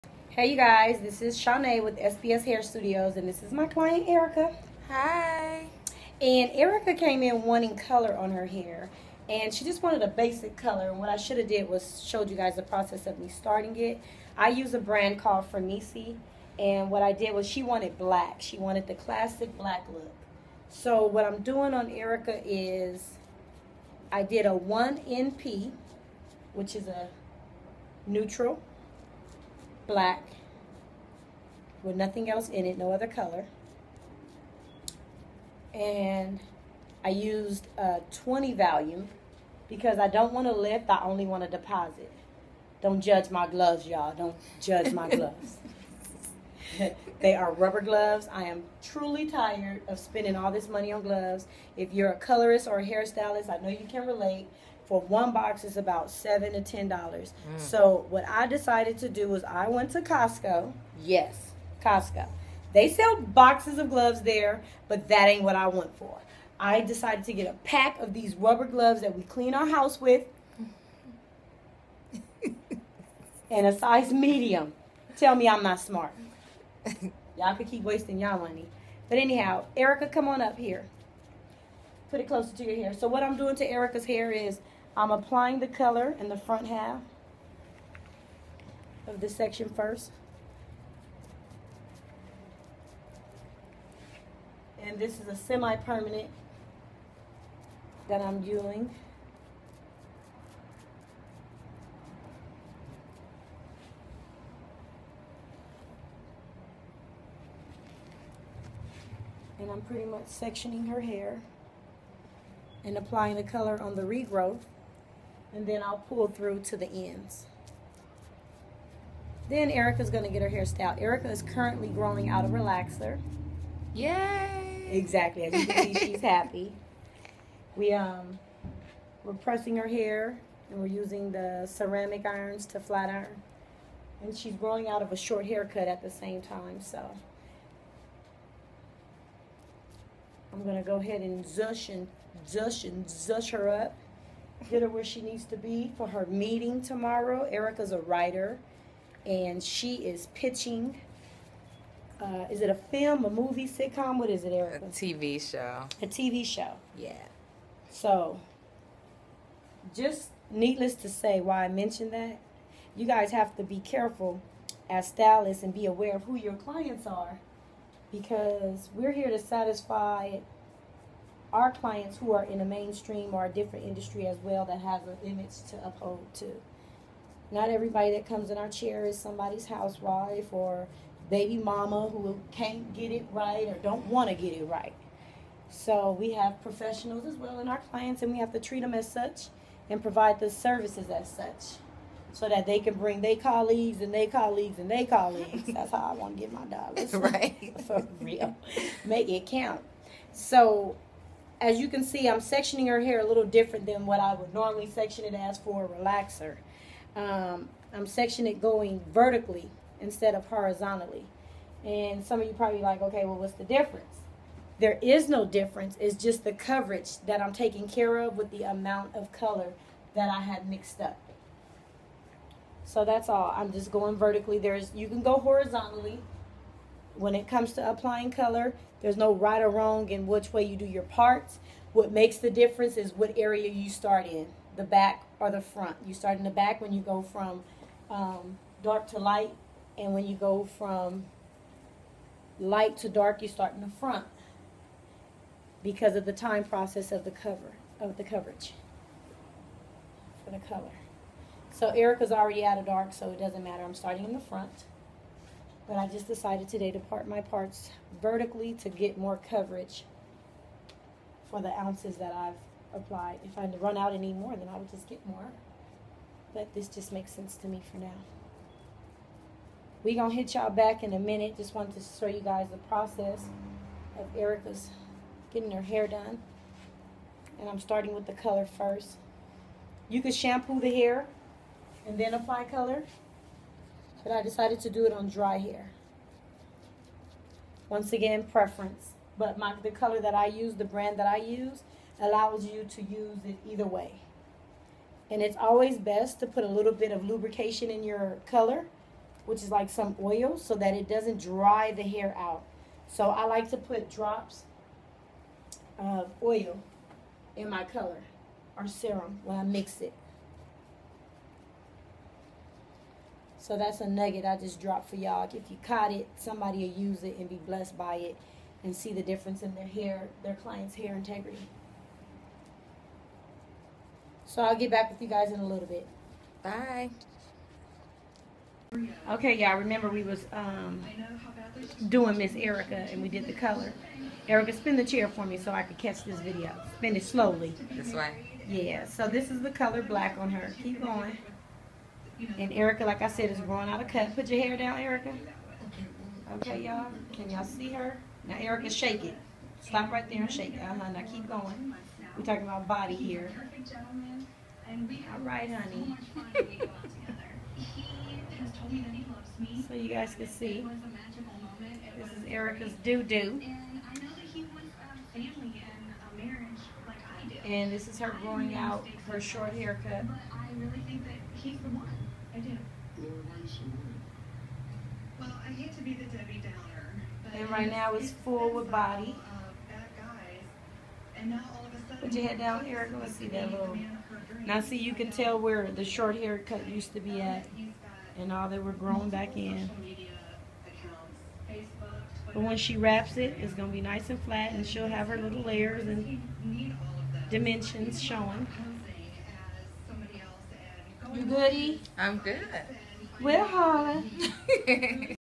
Color by Framesi Styles by Shawne' playing in color a shade of black
I switched color brands from Wella to Framesi. Wella wasn't cutting it for me anymore. The color would be so dull and non vibrant so I decided to try Framesi. OMG it gives me what I need and I'm excited to use it. I play with color frequently and up sale based on my excitement. The decolonization mix is awesome especially when I add Olapex. This color process was just tone on tone